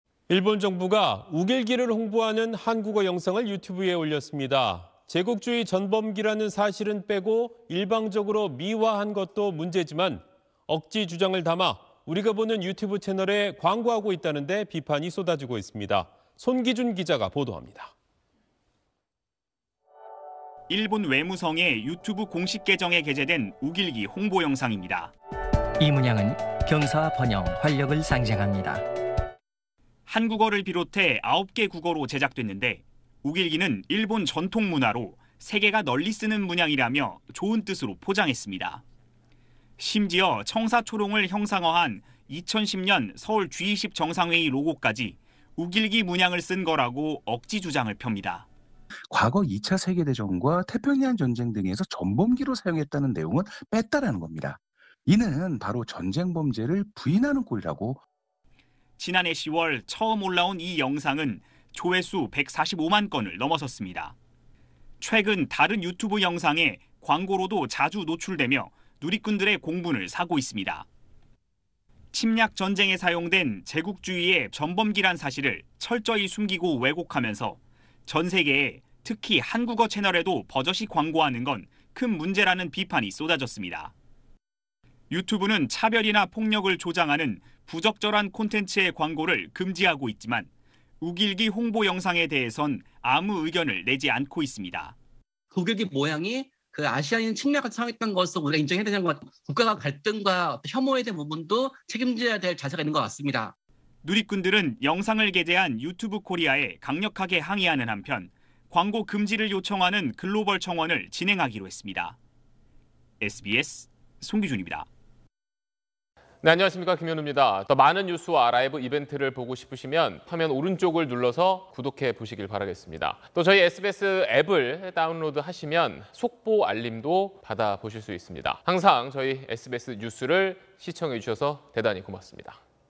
청사초롱 본뜬 로고도 욱일기? 공분 부른 '한국어 광고' / SBS
일본 정부가 욱일기를 홍보하는 한국어 영상을 유튜브에 올렸습니다. '제국주의 전범기'라는 사실은 빼고 일방적으로 미화한 것도 문제지만, 억지 주장을 담아 우리가 보는 유튜브 채널에 광고하고 있다는 데 비판이 쏟아지고 있습니다.
손기준 기자가 보도합니다.